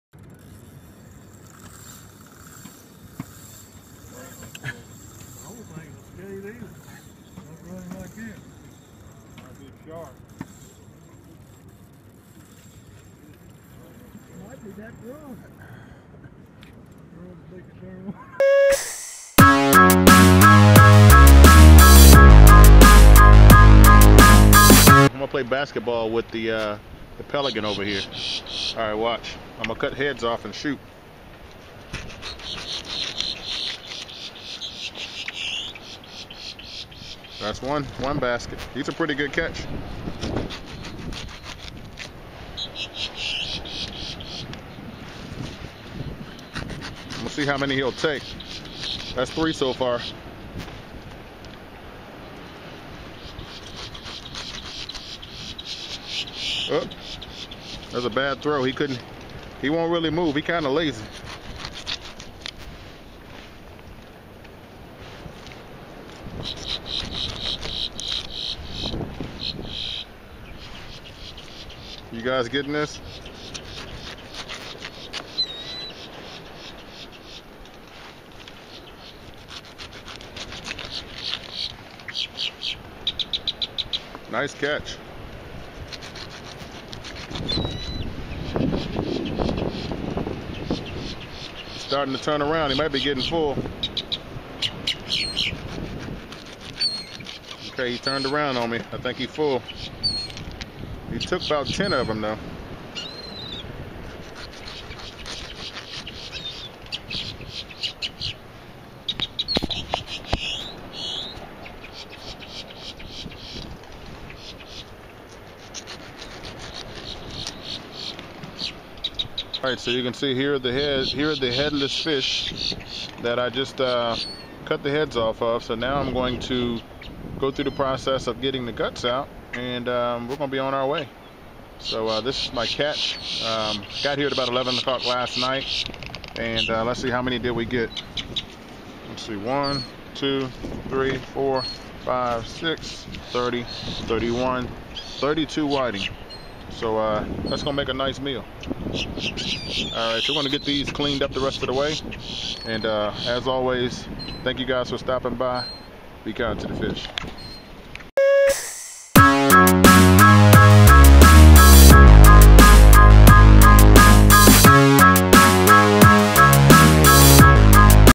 Pelican Catches ~10 Fish Heads in Mouth Wilmington North Carolina
Happy Thanksgiving... The Circle of life is in full effect. One man's waste is a Pelican's dinner. :)